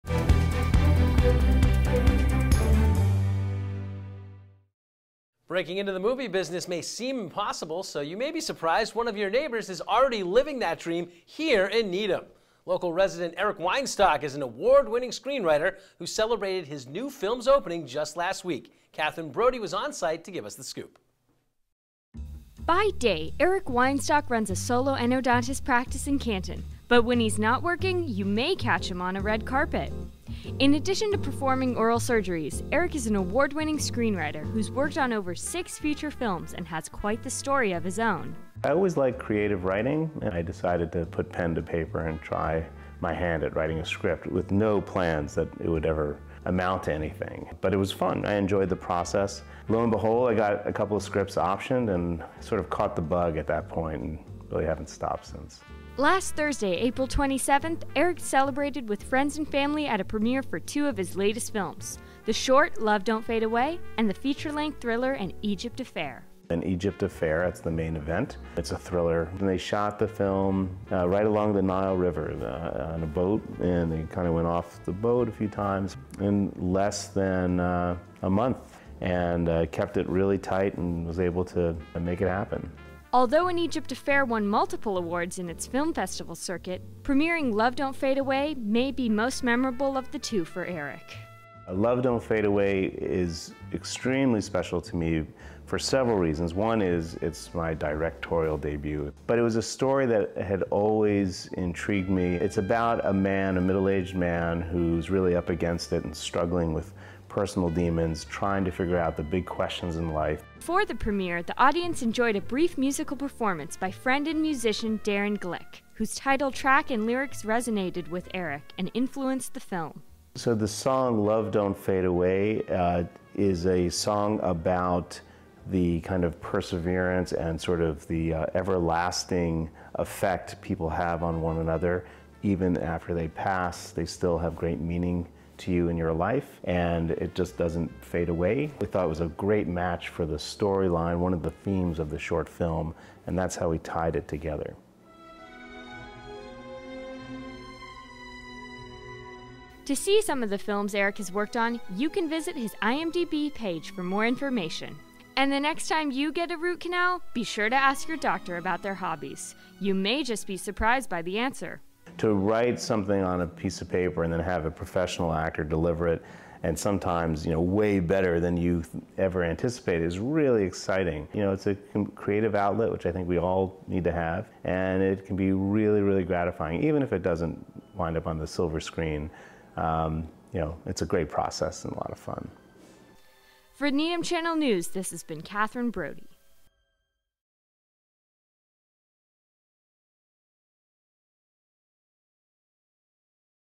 An Egypt Affair & Love Don't Fade Away Premiere
A short and a feature length film a Needham resident wrote premiered recently in Newton.